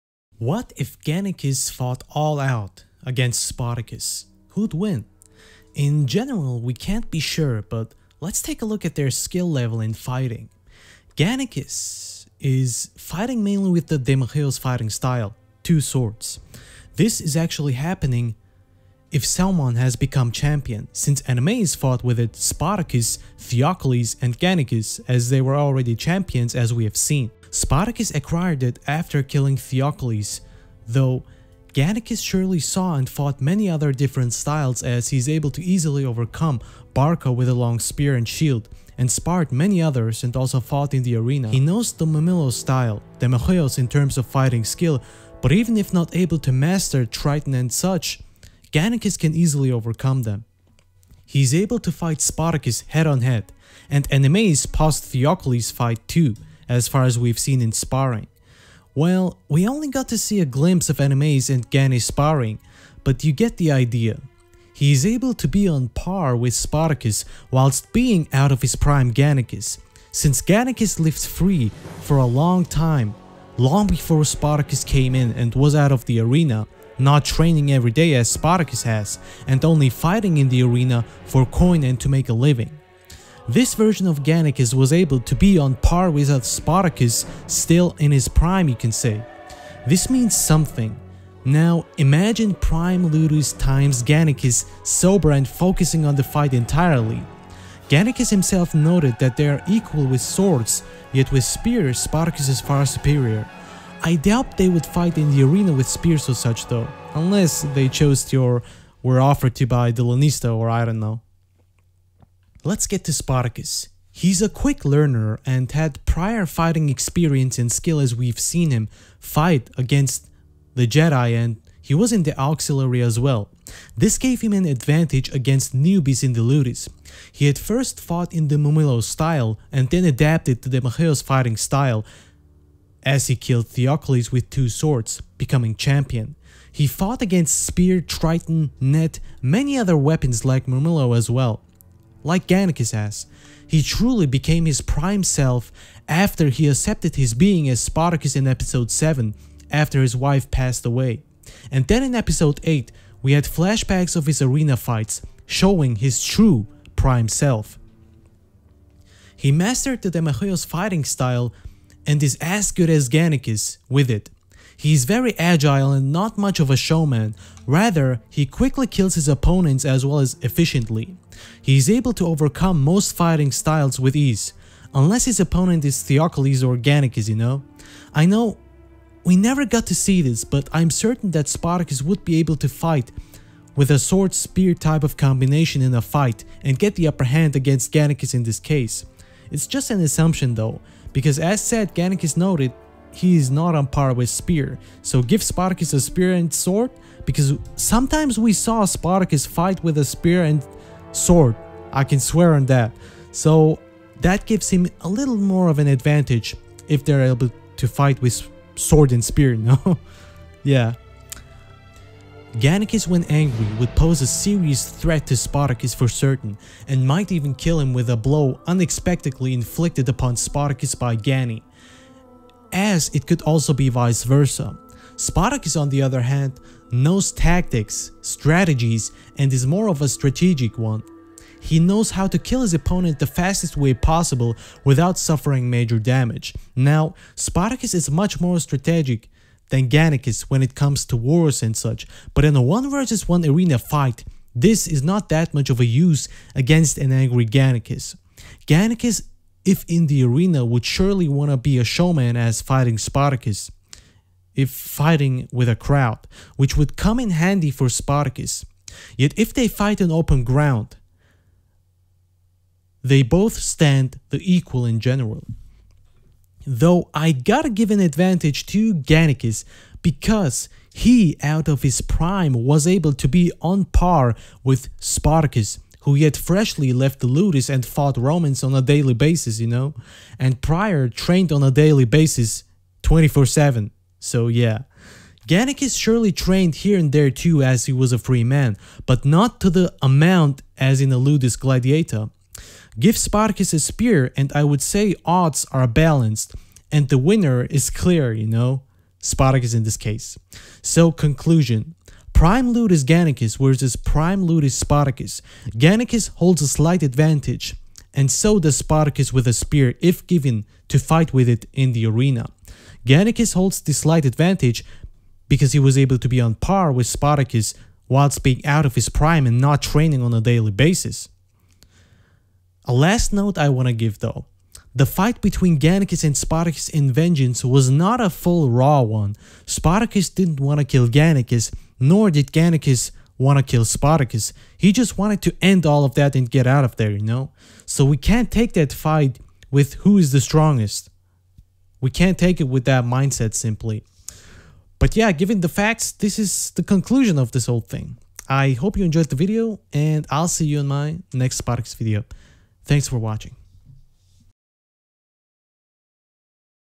Spartacus Vs Gannicus - Who Wins ?
We got to see a really good fight between the two in Vengeance, yet it was not purely 100 % Raw. Gannicus did not want to kill Spartacus, nor did Spartacus wanna kill Gannicus.

In certain scenarios, both Gannicus and Spartacushave their own advantages when fighting each other, depending on those, their winning chances will vary. Overall, they are equal.